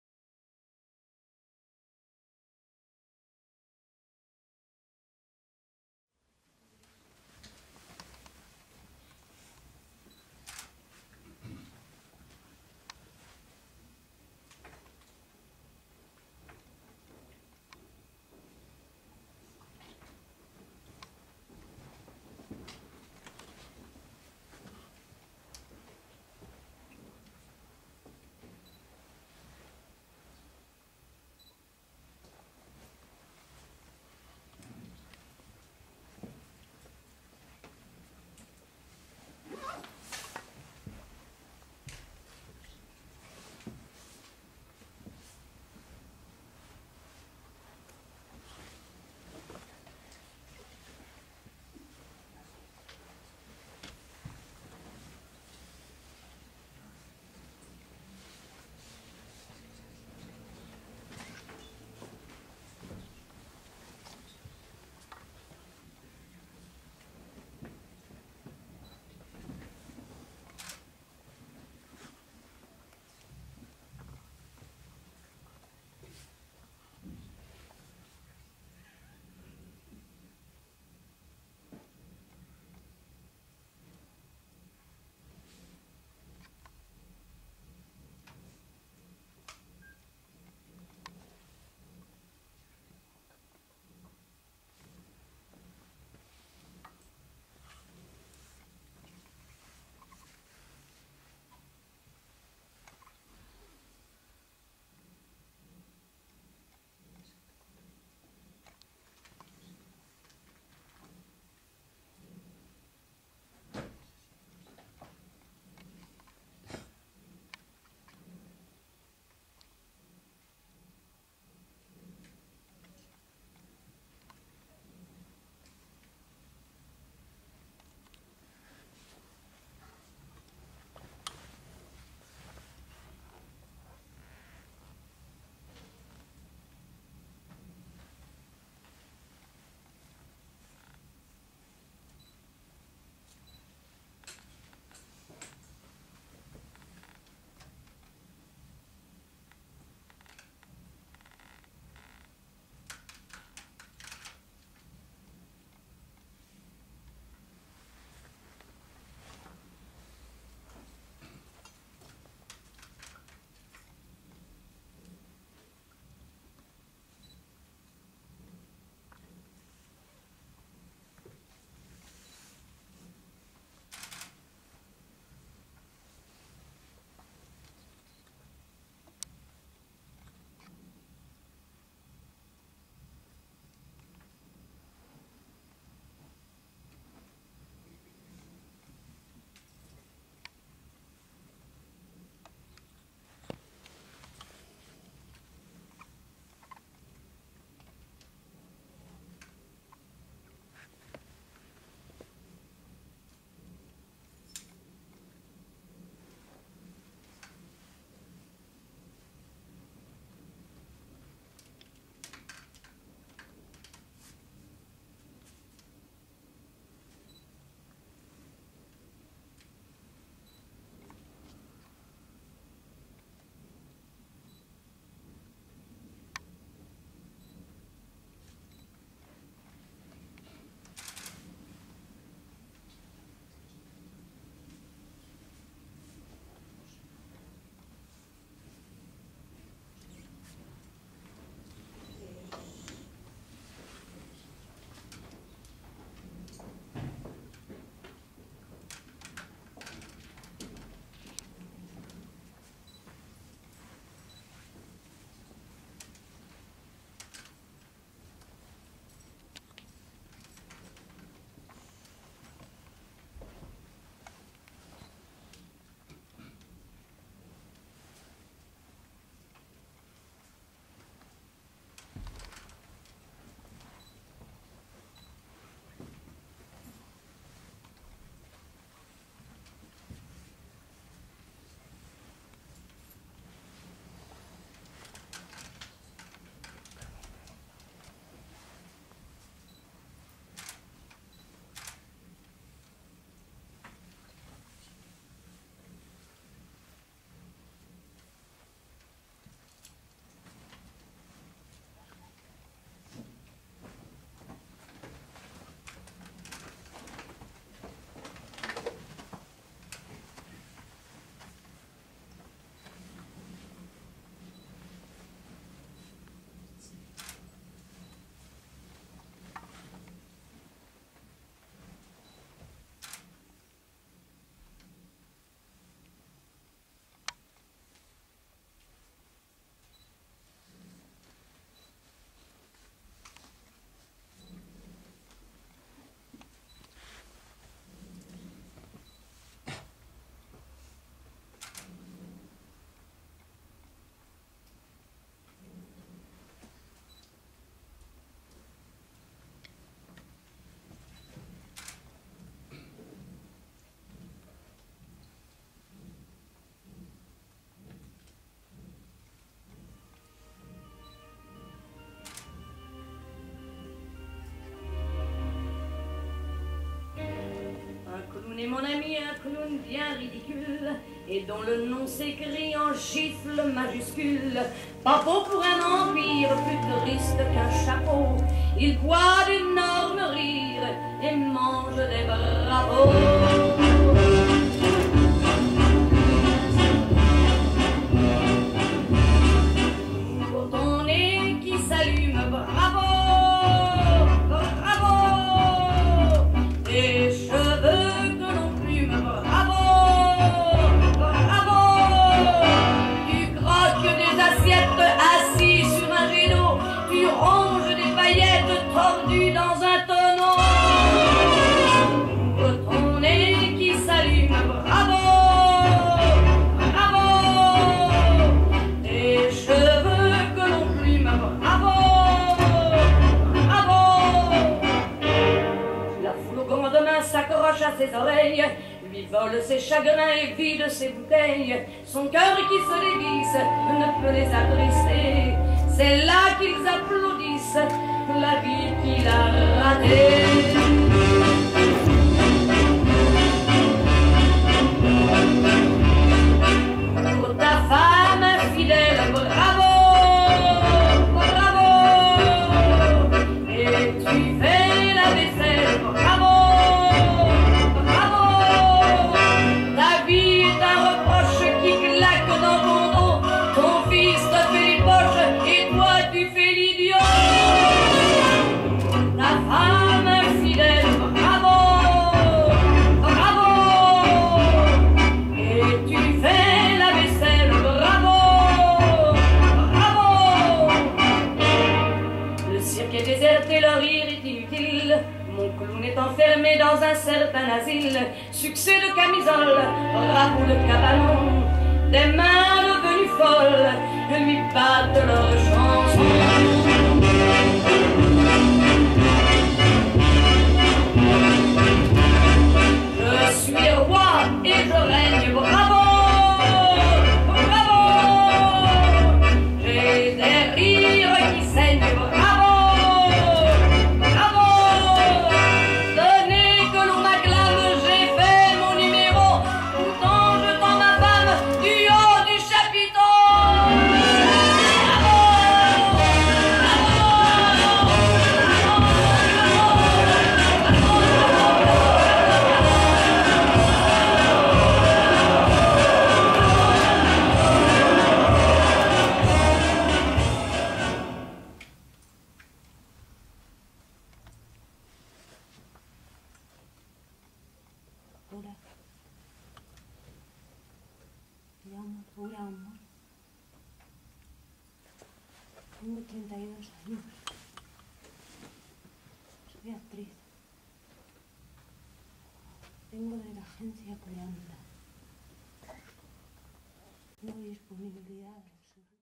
Danza Butoh - Laura Amor - Alfa Institut - Movimiento y Expresión Corporal
Composición creada en interpretada por Laura Amor, presentada en Alfa Institut.